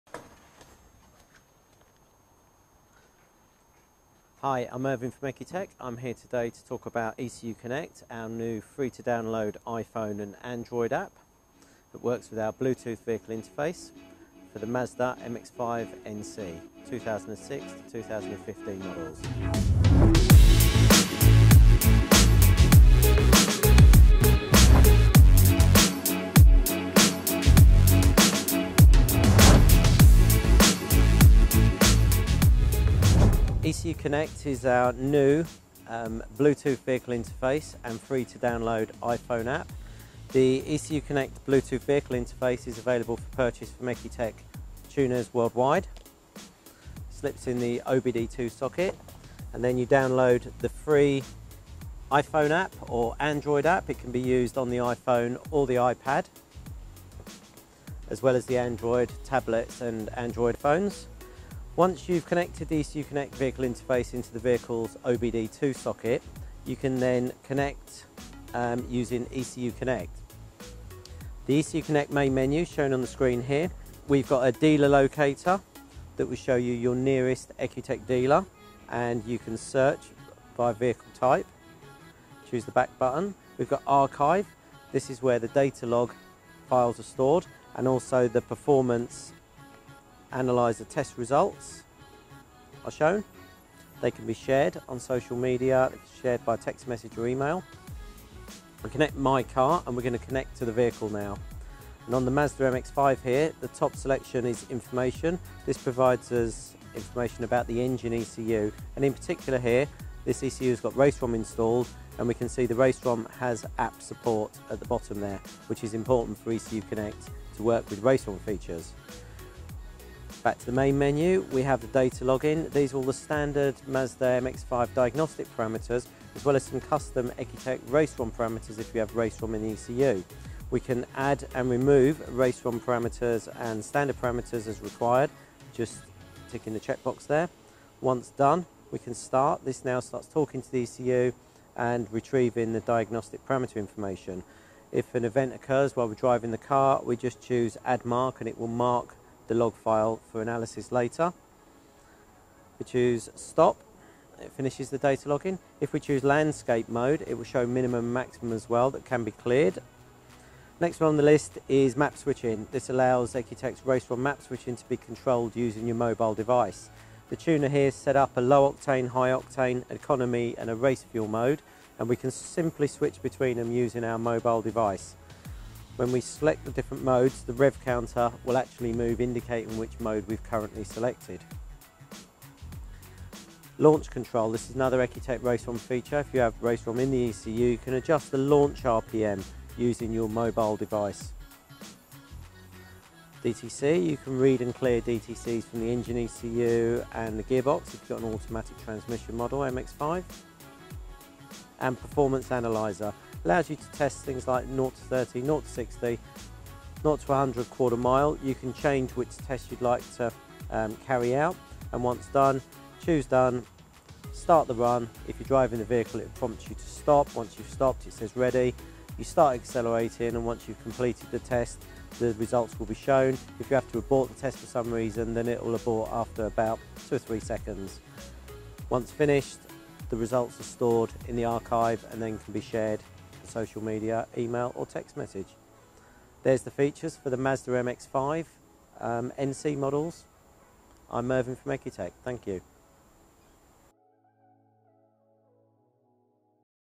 EcuTek ECU Connect and Bluetooth Vehicle Interface for the Mazda MX5 Miata NC
See how you can control your EcuTek tune from your phone or tablet using our ECU Connect App for iPhone and Android, together with our Bluetooth Vehicle Interface